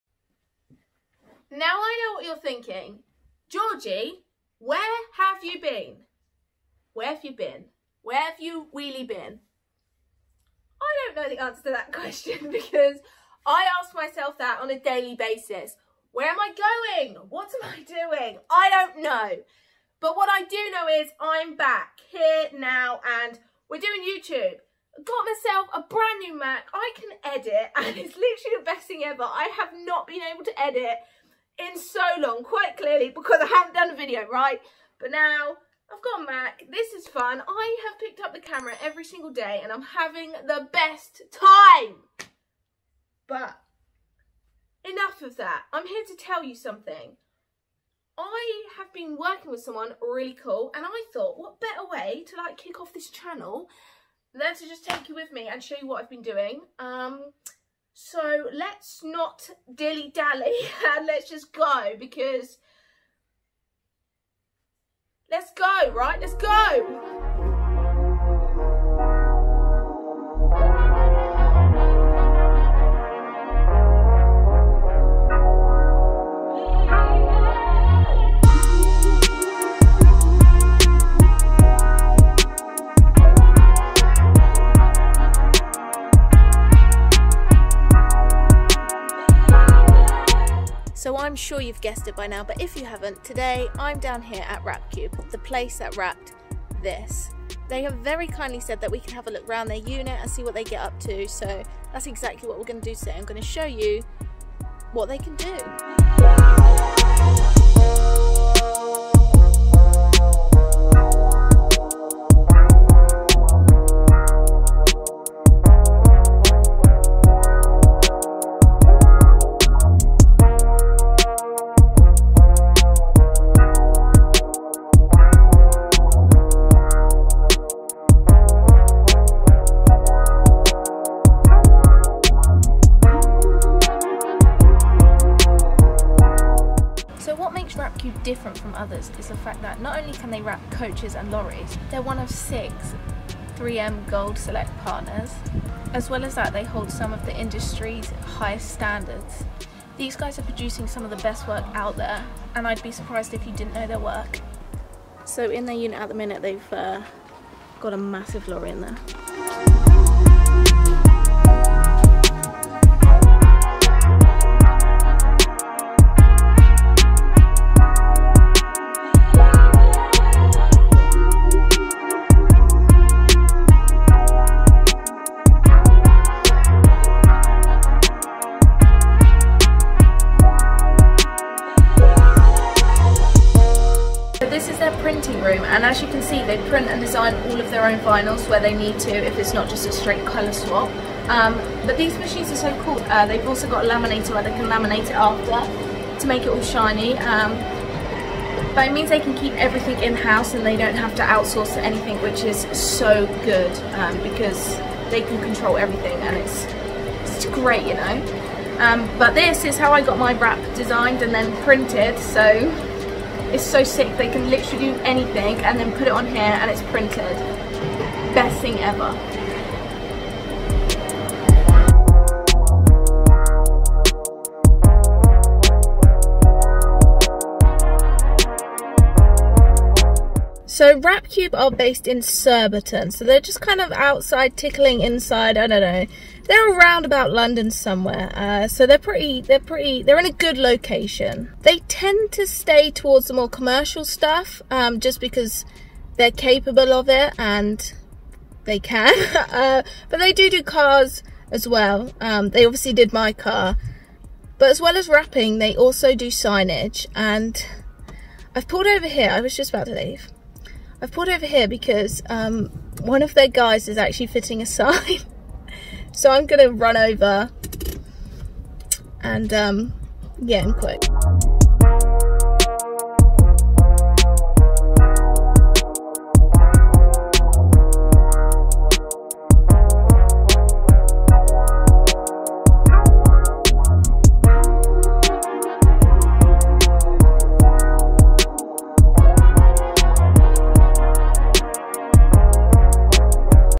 Wrap Cube INSIDE the WRAP SHOP Behind The Wrap Pt1
We went behind the scene at Wrap Cube, one of the UK biggest and best wrap company.
Wrap cube is a 3M gold select partner and has some of the industries highest standards when it comes to wrapping. they have some of the biggest clients in the UK.

Wrap Cube is based in Surbiton, London you can find them on there social media below